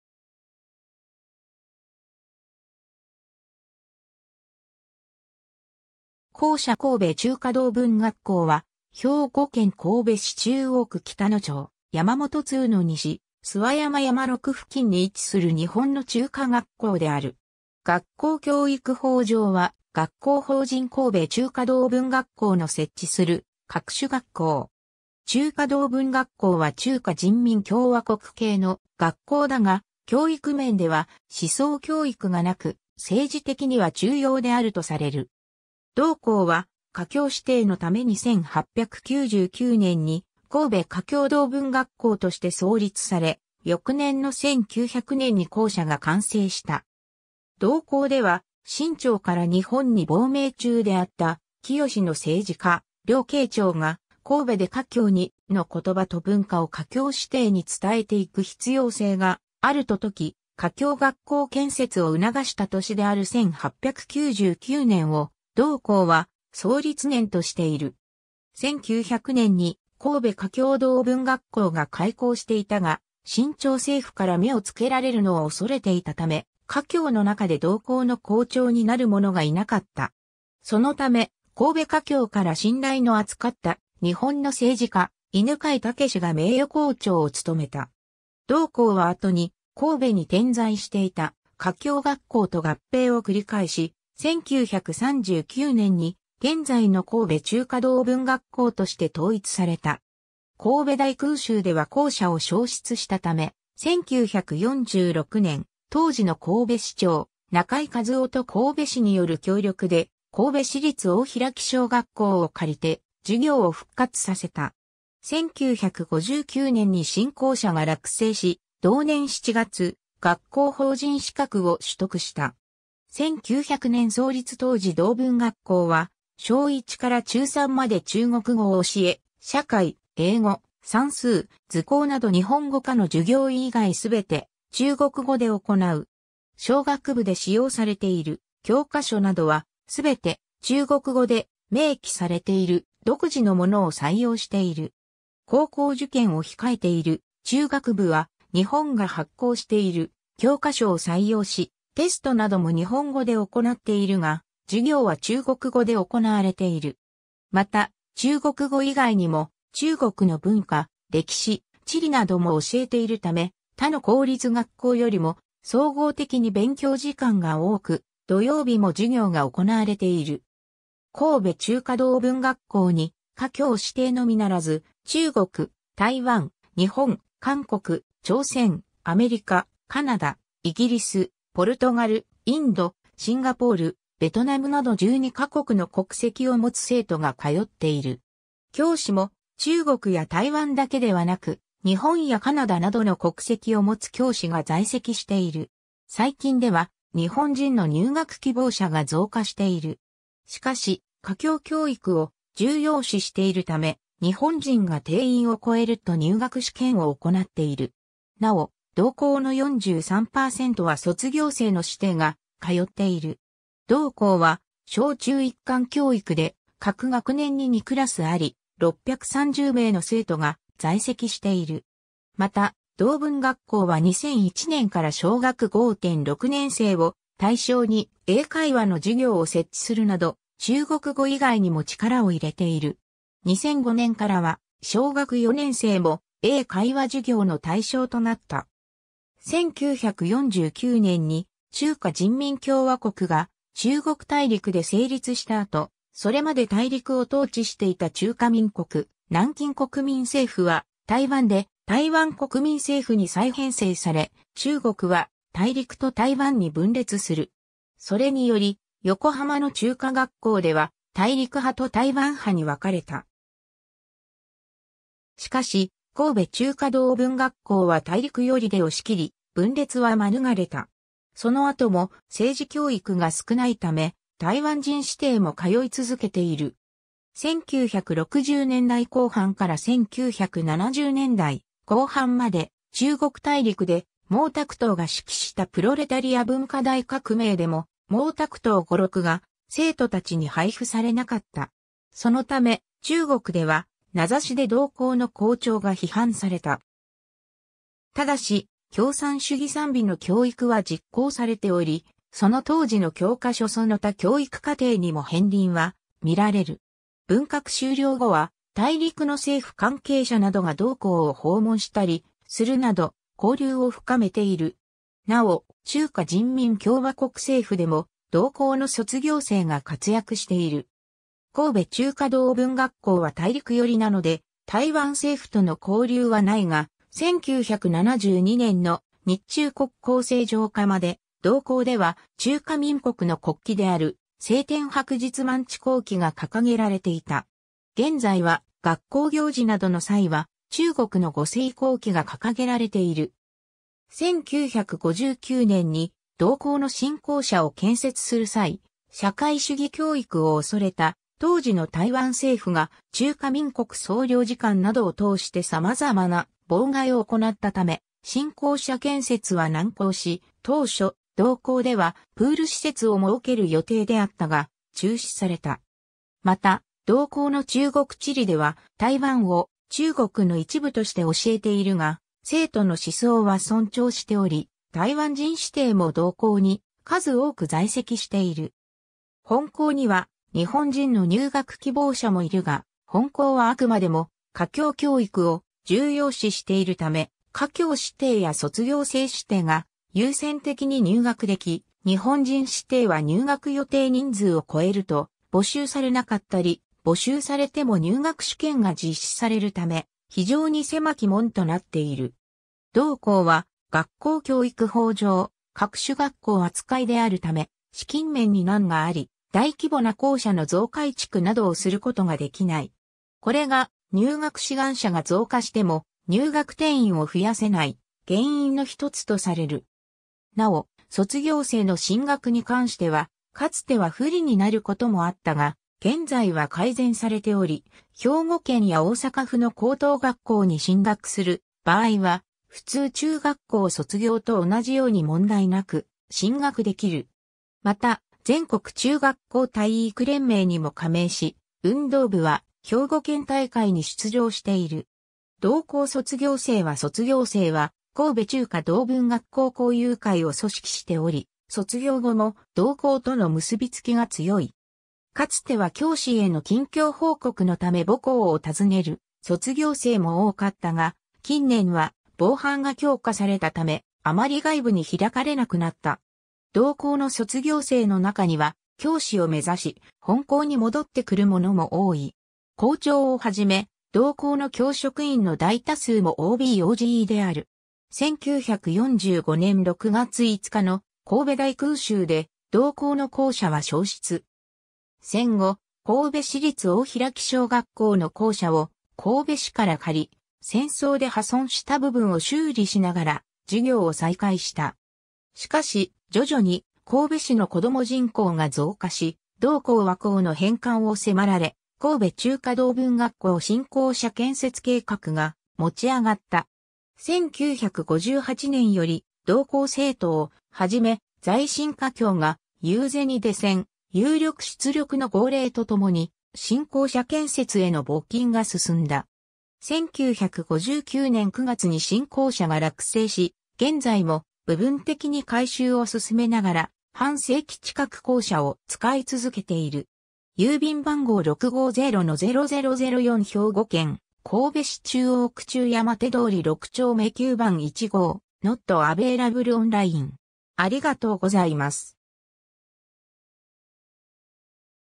神戸中華同文学校
校舎 神戸中華同文学校（こうべちゅうかどうぶんがっこう）は兵庫県神戸市中央区北野町山本通の西、諏訪山山麓付近に位置する日本の中華学校である。
 学校教育法上は学校法人神戸中華同文学校の設置する各種学校。
中華同文学校は中華人民共和国系の学校だが、教育面では思想教育がなく、政治的には中庸であるとされる。
同校は、華僑子弟のために1899年に神戸華僑同文学校として創立され、翌年の1900年に校舎が完成した。
同校では、清朝から日本に亡命中であった清の政治家・梁啓超が神戸で華僑に、(中国)の言葉と文化を華僑子弟に伝えて行く必要性があると説き、華僑学校建設を促した年である1899年を、同校は創立年としている。
1900年に神戸華僑同文学校が開校していたが、清朝政府から目をつけられるのを恐れていたため、華僑の中で同校の校長になる者がいなかった。
そのため、神戸華僑から信頼の厚かった日本の政治家・犬養毅が名誉校長を務めた。
同校は後に、神戸に点在していた華僑学校と合併を繰り返し、1939年に現在の神戸中華同文学校として統一された。
神戸大空襲では校舎を焼失したため、1946年、当時の神戸市長中井一夫と神戸市による協力で神戸市立大開小学校を借りて、授業を復活させた。
1959年に新校舎が落成し、同年7月、学校法人資格を取得した。
 1900年創立当時 同文学校は小1から中3まで中国語を教え、社会、英語、算数、図工など日本語科の授業以外全て中国語で行う。
小学部で使用されている教科書などは、全て中国語で明記されている独自のものを採用している。
高校受験を控えている中学部は、日本が発行している教科書を採用し、テストなども日本語で行っているが、授業は中国語で行われている。
また、中国語以外にも中国の文化、歴史、地理なども教えているため、他の公立学校よりも総合的に勉強時間が多く、土曜日も授業が行われている。
 神戸中華同文学校に華僑子弟のみならず、中国、台湾（中華民国）、日本、韓国、朝鮮、アメリカ、カナダ、イギリス、ポルトガル、インド、シンガポール、ベトナムなど12カ国の国籍を持つ生徒が通っている。
教師も、中国や台湾だけではなく日本やカナダなどの国籍を持つ教師が在籍している。
最近では、日本人の入学希望者が増加している。
しかし、華僑教育を重要視しているため、日本人が定員を超えると入学試験を行っている。
なお、同校の43%は卒業生の子弟が通っている。
同校は小中一貫教育で各学年に2クラスあり、630名の生徒が在籍している。
また、同文学校は2001年から小学5・6年生を対象に英会話の授業を設置するなど、中国語以外にも力をいれている。
2005年からは、小学4年生も英会話授業の対象となった。
 1949年に中華人民共和国が中国大陸で成立した後、それまで大陸を統治していた中華民国・南京国民政府は台湾で台湾国民政府に再編成され、中国は大陸と台湾に分裂する。
それにより、横浜の中華学校では大陸派と台湾派に分かれた。
しかし、神戸中華同文学校は大陸寄りで押し切り、分裂はまぬがれた。
その後も政治教育が少ないため、台湾人子弟も通い続けている。
1960年代後半から1970年代後半まで中国大陸で、毛沢東が指揮したプロレタリア文化大革命でも毛沢東語録が生徒達に配布されなかった。
そのため、中国では名指しで同校の校長が批判された。
ただし、共産主義讃美の教育は実行されており、その当時の教科書その他教育課程にも片鱗は見られる。
文革終了後は大陸の政府関係者などが同校を訪問したりするなど、交流を深めている。
なお、中華人民共和国政府でも、同校の卒業生が活躍している。
 神戸中華同文学校は大陸寄りなので、台湾（中華民国）政府との交流はないが、1972年の日中国交正常化まで、同校では中華民国の国旗である青天白日満地紅旗が掲げられていた。
現在は学校行事などの際は中国の五星紅旗が掲げられている。
 1959年に同校の新校舎を建設する際、社会主義教育を恐れた当時の台湾政府が中華民国総領事館などを通してさまざまな妨害を行ったため新校舎建設は難航し、当初、同校ではプール施設を設ける予定であったが、中止された。
 また、同校の中国地理では台湾を中国の一部として教えているが、生徒の思想は尊重しており、台湾人子弟も同校に数多く在籍している。
 本校には日本人の入学希望者もいるが、本校はあくまでも華僑教育を重要視しているため、華僑子弟や卒業生子弟が優先的に入学でき、日本人子弟は入学予定人数を超...